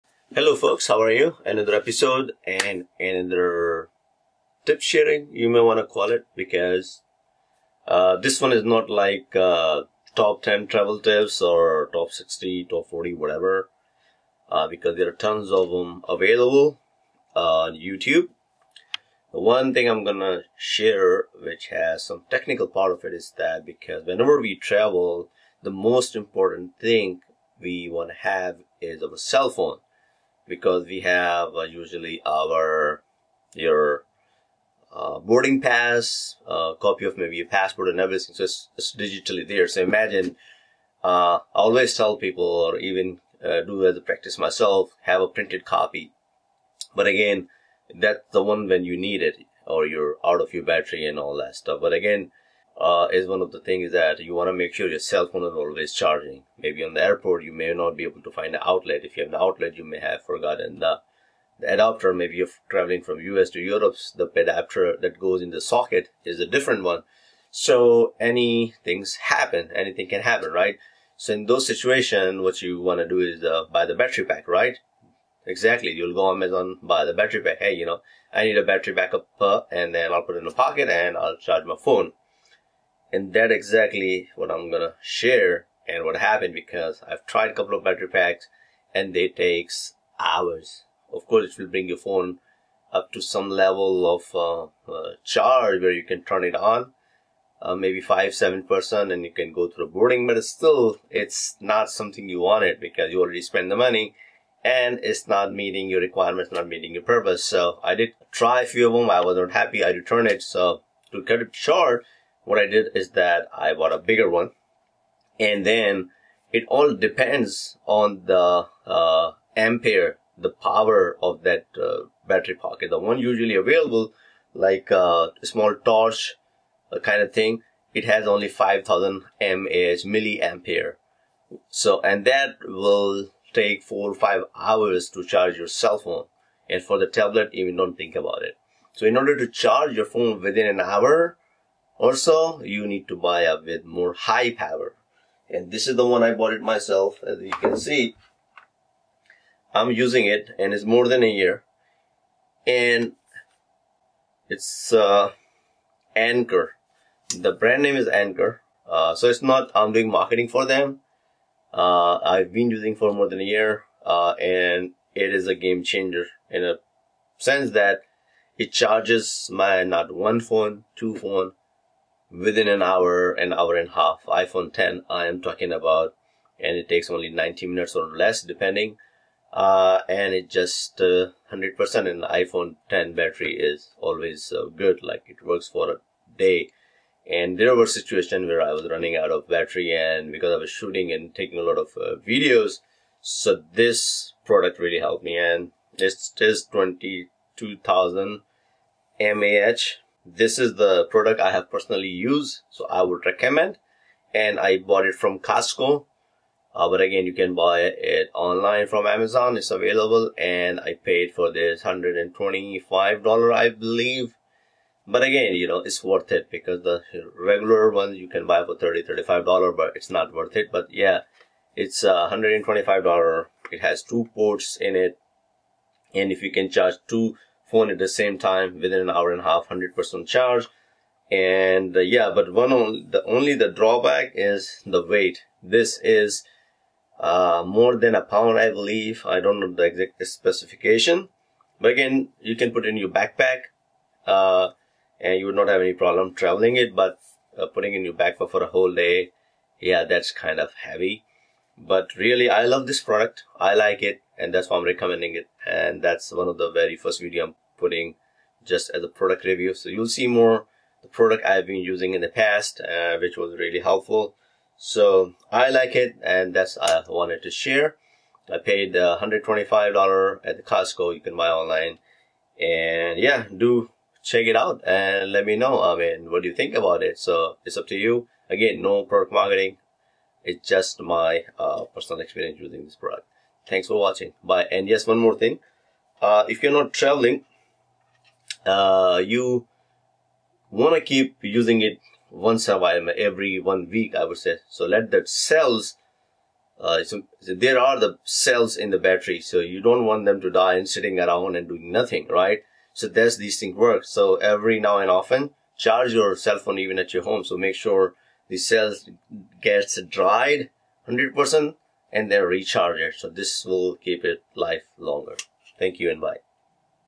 iPhone charges in 60 minutes
Here is my experience with Anker Power Bank. I bought it last year and took it several times on my various. If you are interested in buying a Power Bank check out this video.